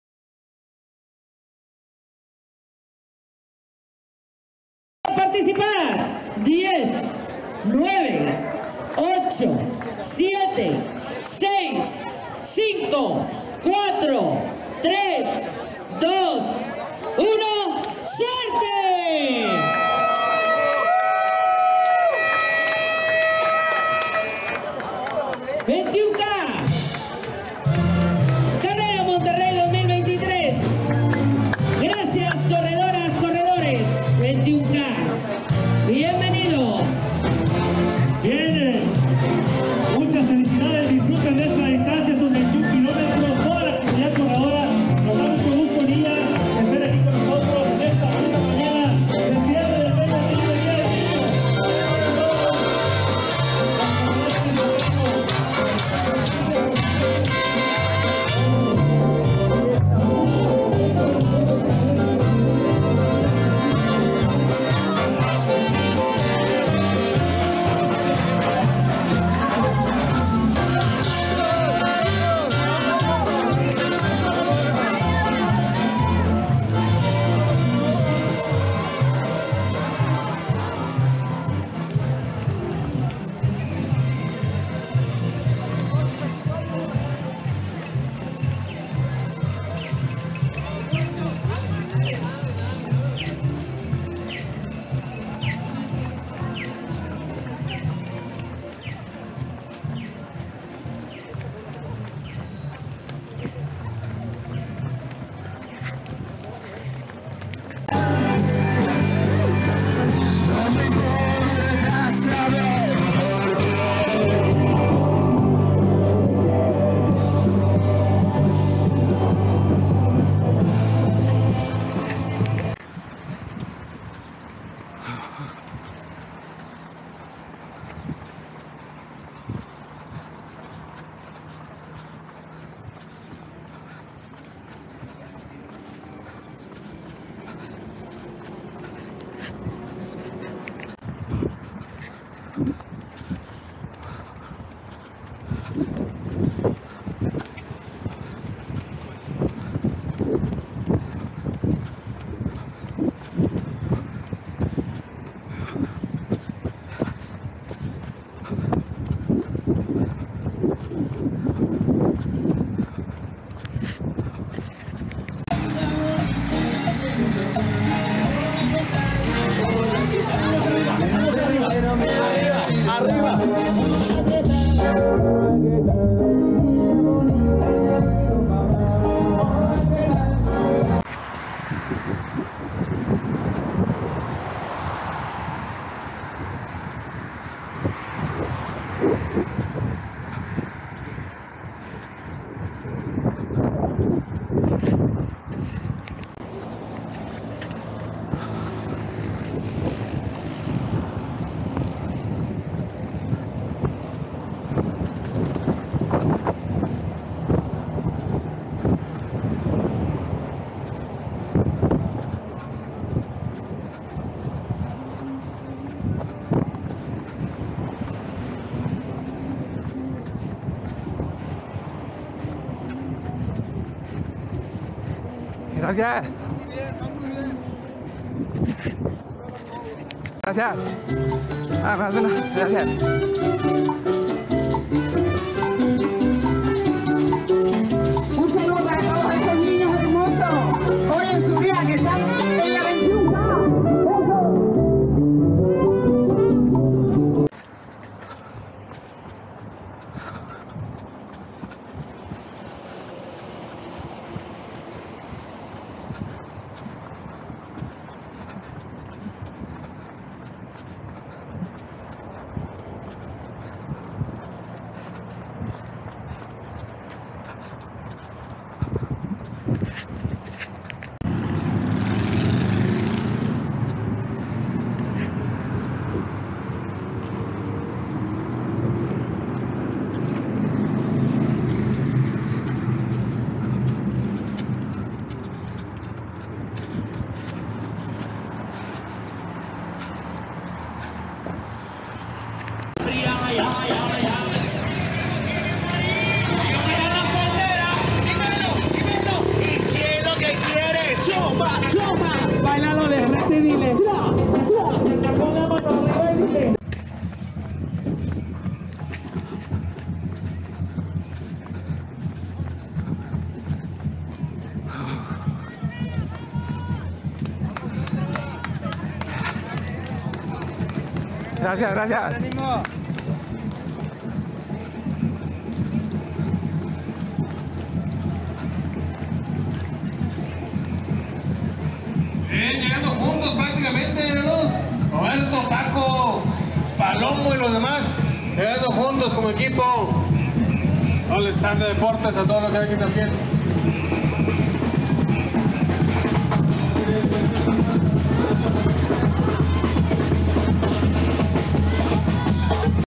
21K MONTERREY 2023
21k Monterrey 2023 llevado a cabo el 30 de abril de 2023 en Nuevo León, México.